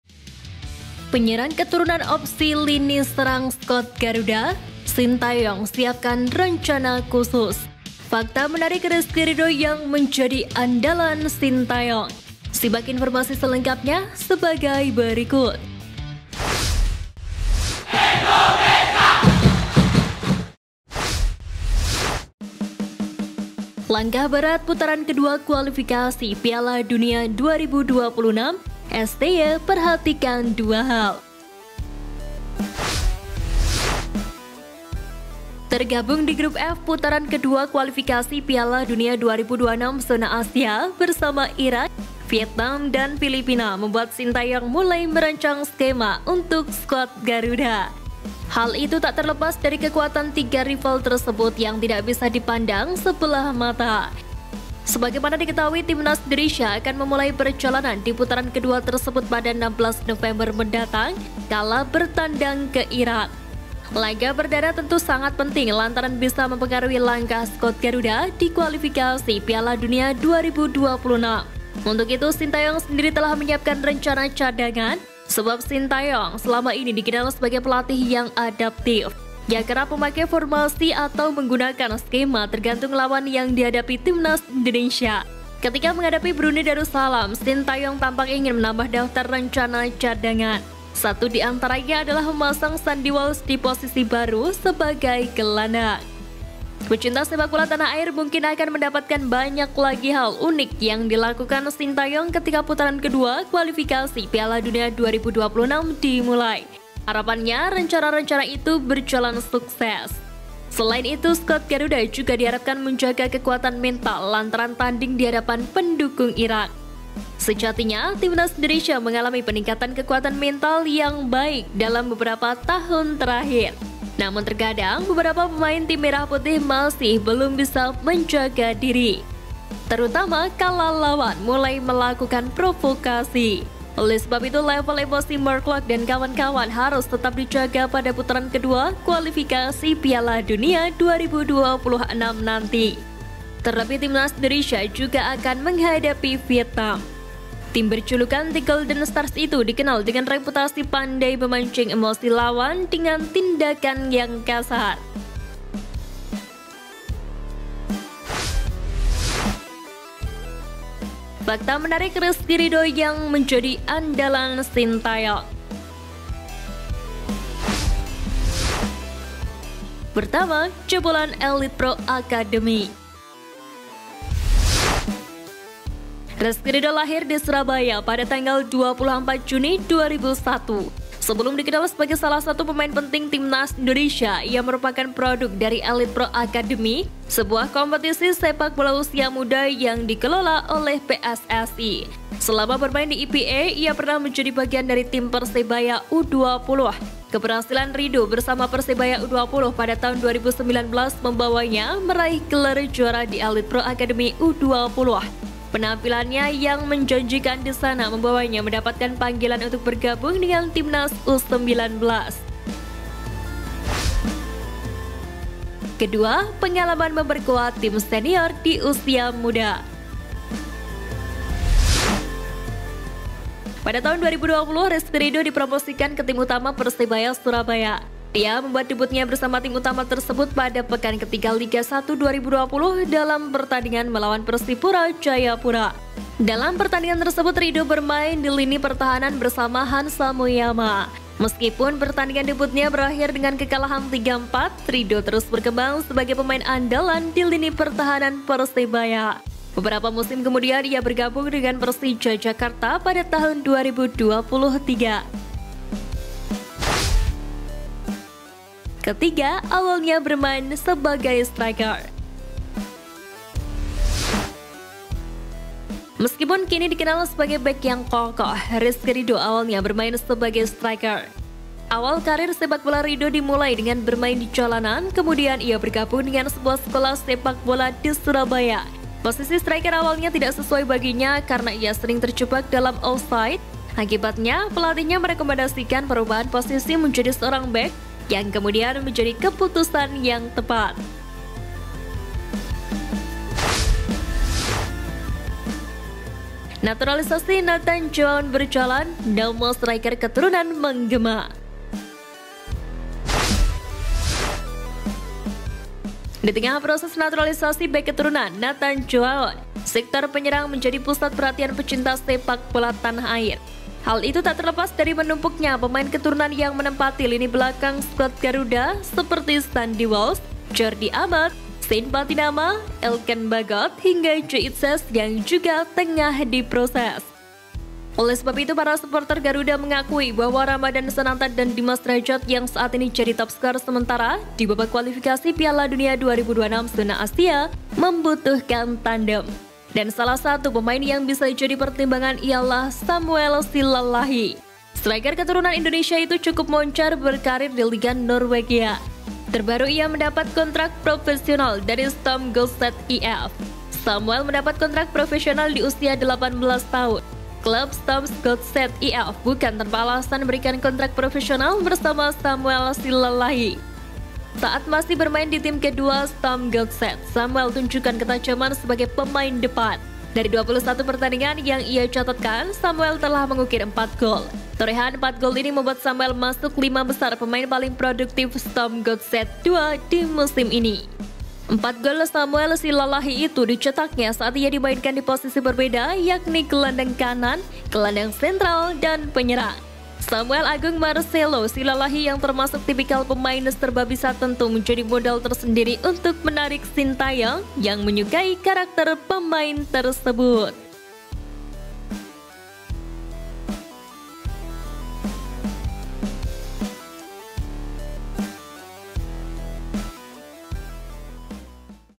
PERMINTAAN DITURUTI! Pagi2 Striker Keturunan menggema demi Timnas~STY rancang 2 skema sekaligus
BERITA BOLA TERBARU DAN TERUPDATE DARI GARUDA SPACE - Berita Bola Hari Minggu, 22 Oktober 2023 : PERMINTAAN DITURUTI! Pagi2 Striker Keturunan menggema demi Timnas~STY rancang 2 skema sekaligus